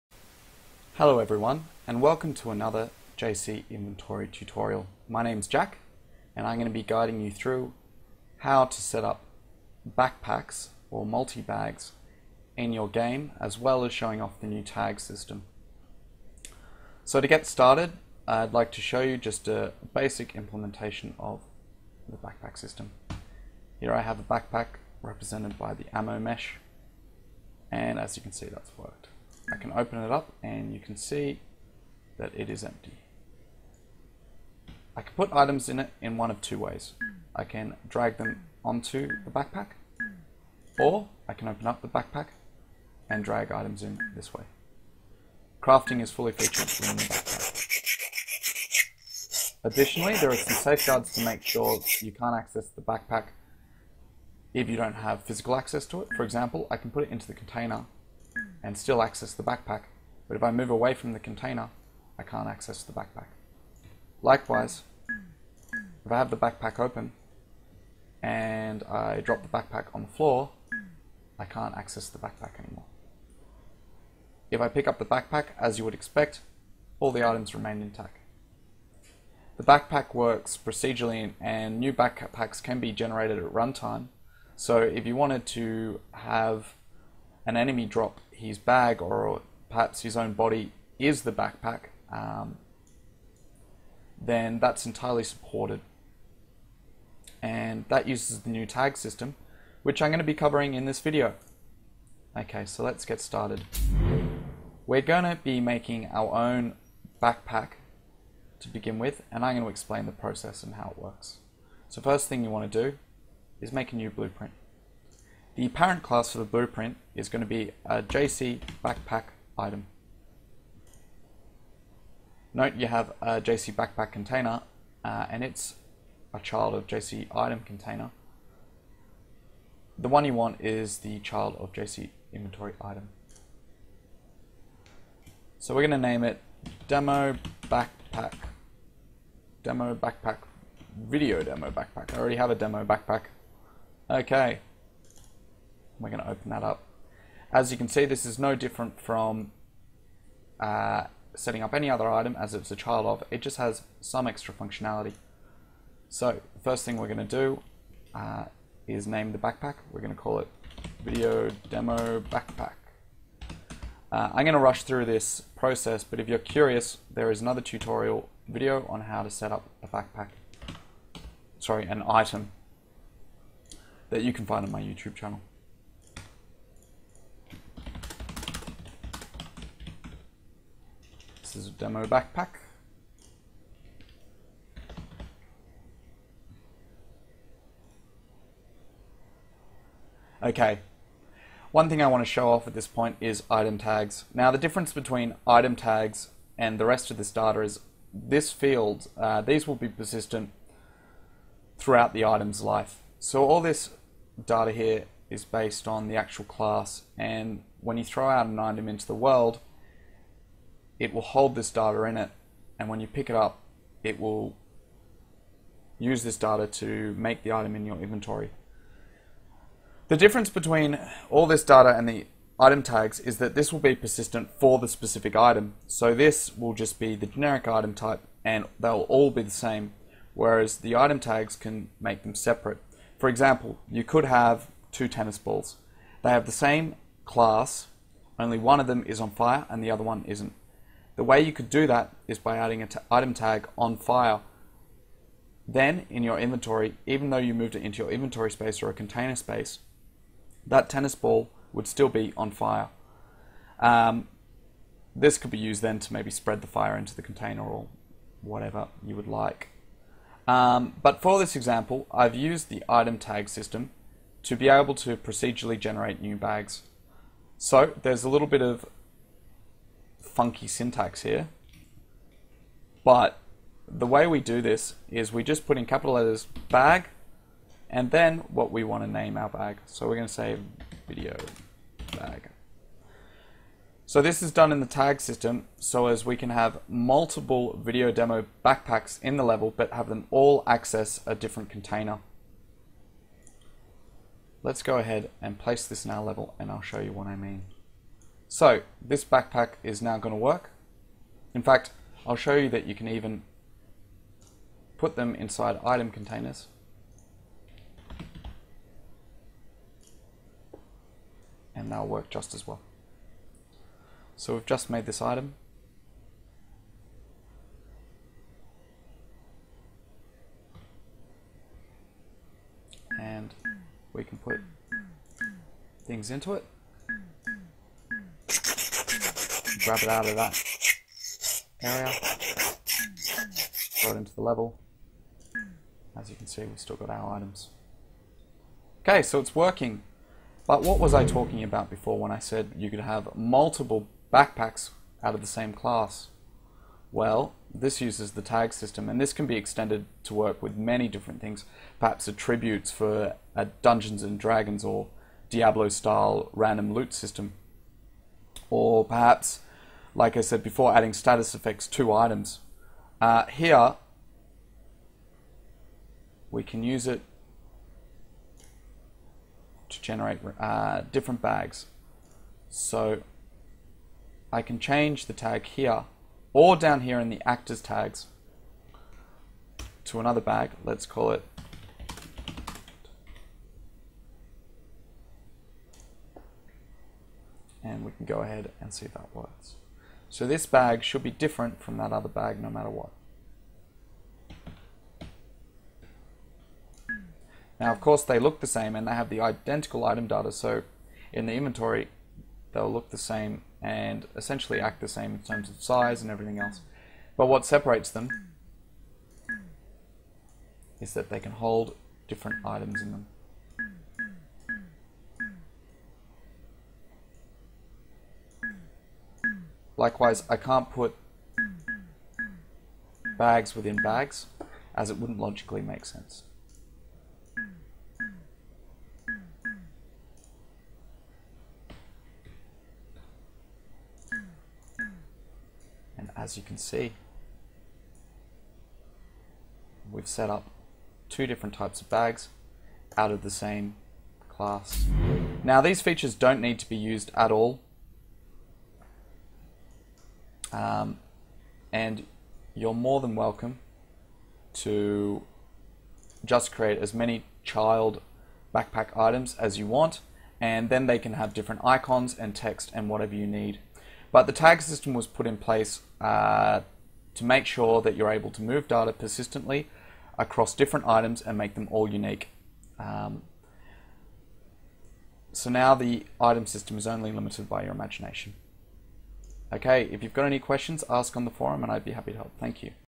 JCInventory Backpack and Tag tutorial
Quick demo and tutorial for the backpack system for JCInventory,